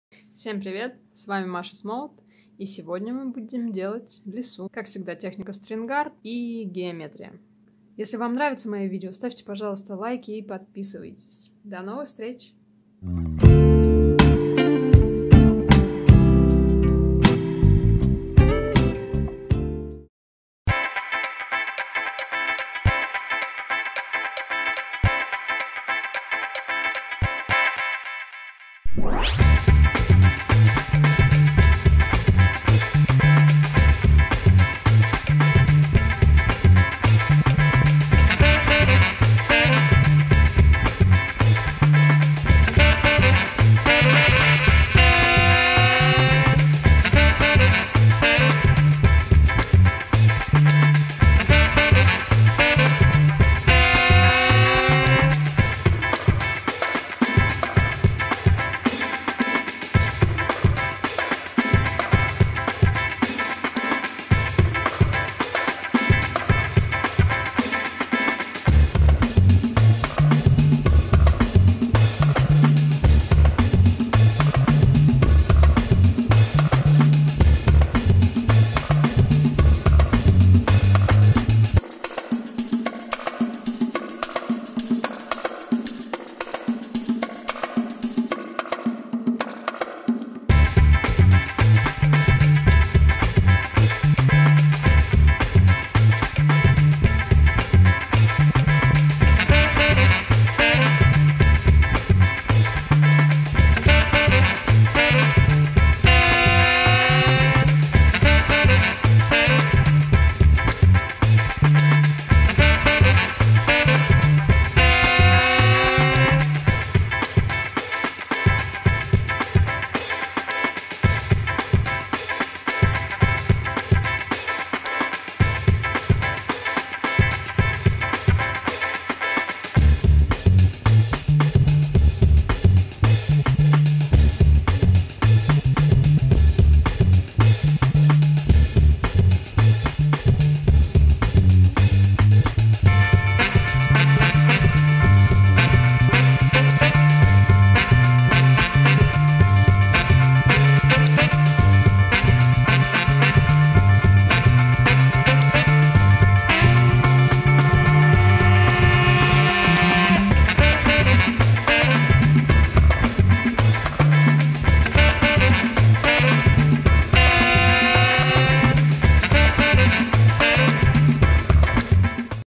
Создание картины в стиле СТРИНГАРТ / STRINGART
Как создать своими руками лису в стиле стрингарт / stringart.